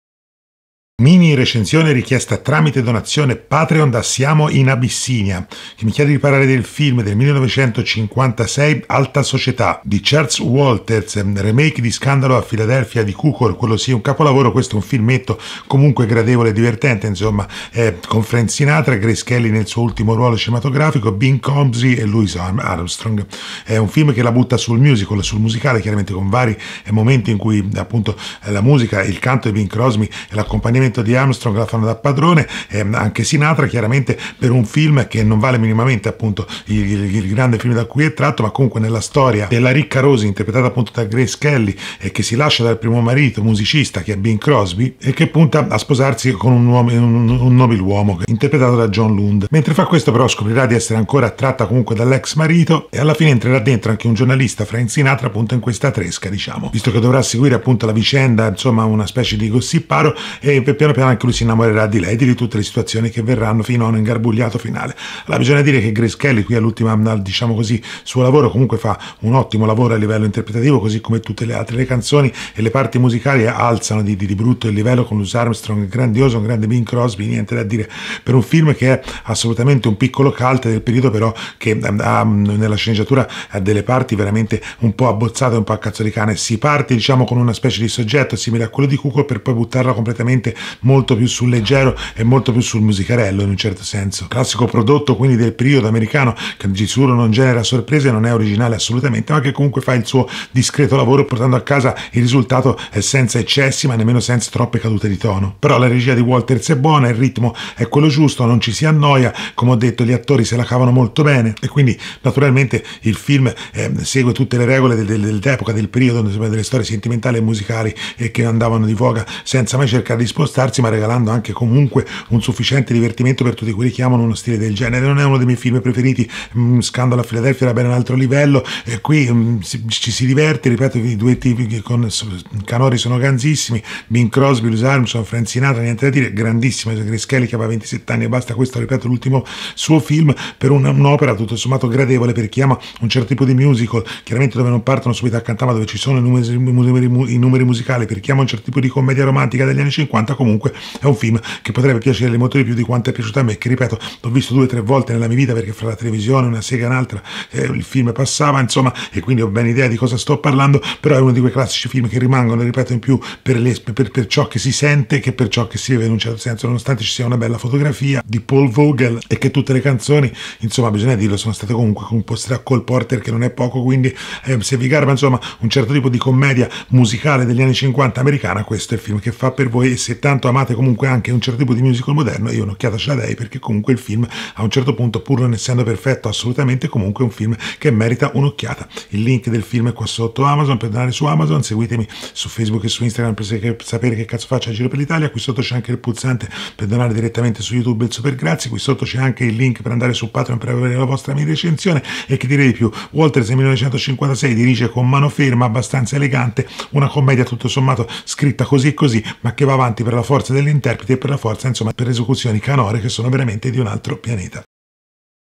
Patreon: Alta società (1956) di Charles Walters - Minirece richiesta da Siamo In Abissinia
▼▼CONTINUA SOTTO▼▼

Lista dei film citati:
Alta società (1956) di Charles Walters